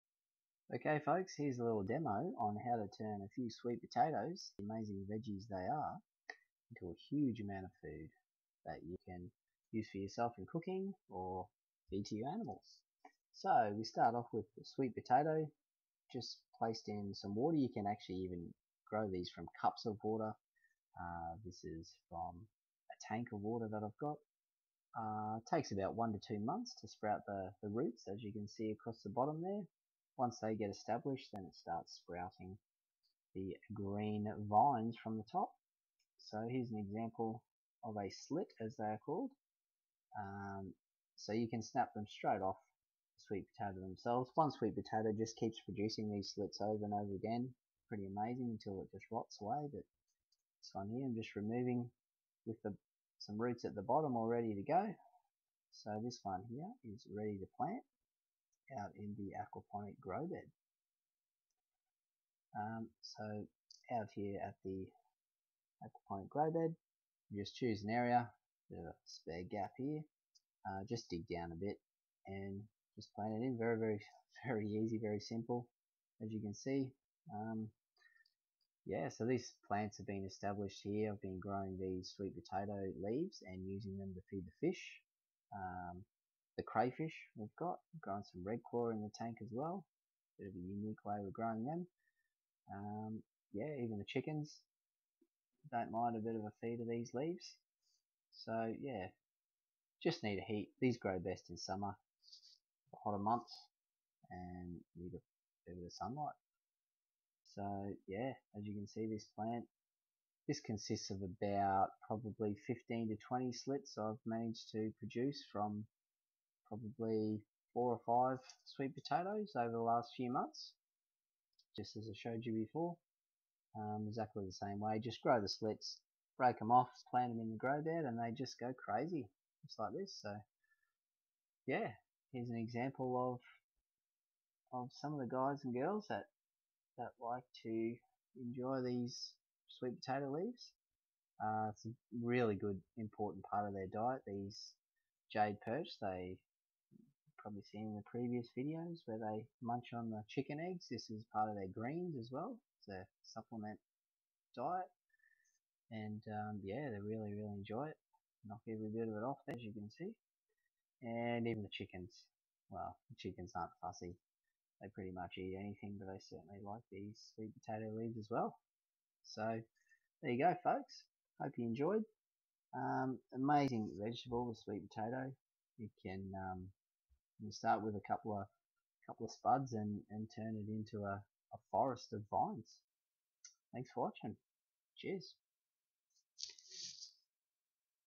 Aquaponic Sweet potato: How to grow Sweet Potato Vines in Aquaponic Systems
This is a short demonstration on how to sprout and grow sweet potato vines/leaves.  Just a few sweet potatoes can produce over a dozen "slits" or sprouts.  Once sprouted, these slits can be planted in the garden, or in this case the aquaponic grow bed, to produce not only sweet potatoes, but hundreds of leaves which make a great food source for fish and other animals.

This really is a powerful vegetable, that, with enough water, heat and sunlight, can produce higher outputs, than its required inputs.